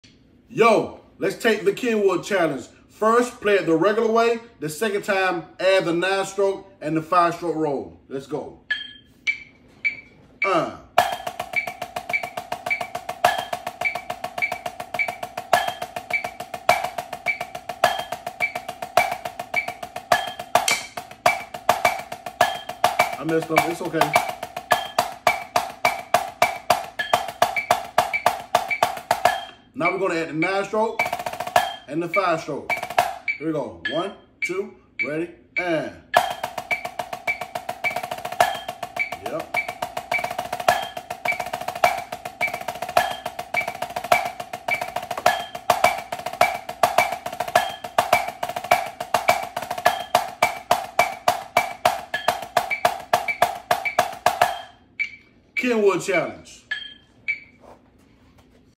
Snare Drum Challenge with Atlanta Drum Academy!
Snare Drum Challenge of the day with Mr.Riles from Atlanta Drum Academy. Grab your snare drum sticks, your practice pad and your metronome and come take this snare drum challenge. Can you read music? If not, just try your best to follow along. Can you play a 9 stroke and a 5 stroke roll? If so, you may just ace this snare drum challenge. This is good for playing 16th notes with accents and diddles. Keep the tempo steady and your sticks at a good height on the rolls (around 6 inches) and 12 inches on the accents. You can do this!!

This excerpt comes from our book entitled "This is how we do it" and it can be purchased